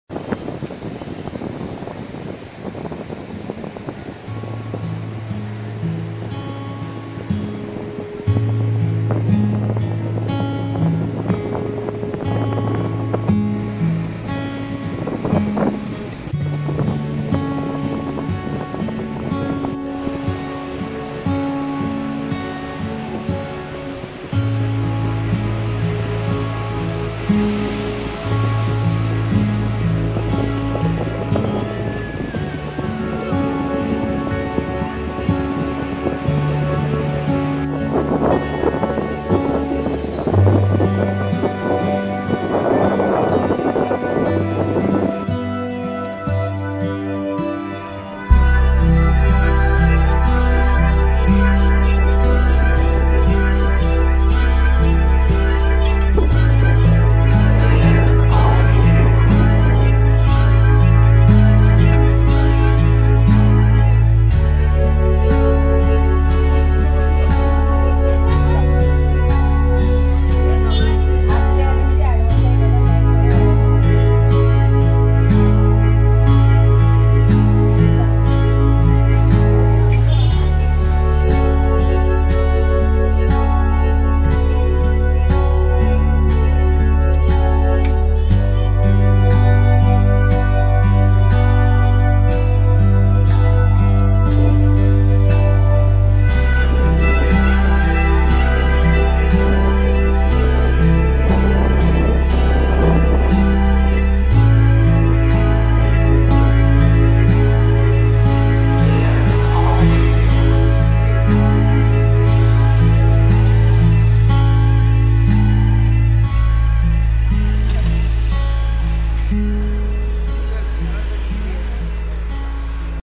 Spend Your Year End In GOA
GOA was a portuguese colony way back in history. With lush green and blue sea shores Goa is a perfect year end destinations for couples and families.
This video was shot this year in Palolem and Anjuna beach.